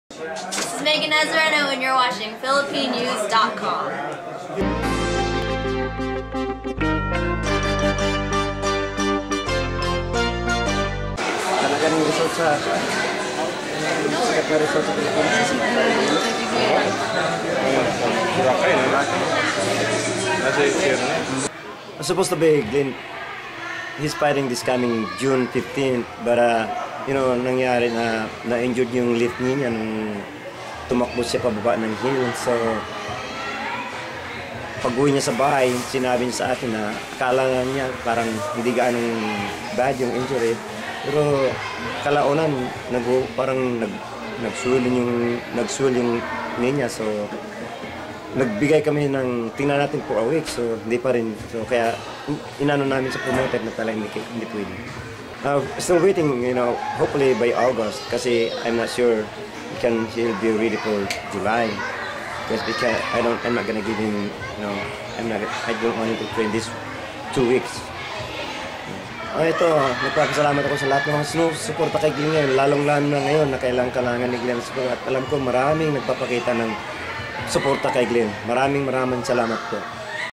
Glenn Donaire recovering from Knee Injury
Boxing Champion Glenn "The Filipino Bomber" Donaire is recovering from his Knee Injury. Donaire, who was suppose to fight last night in Florida, is expect to return to the ring around August.

Glenn Donaire
Nonito Donaire Sr.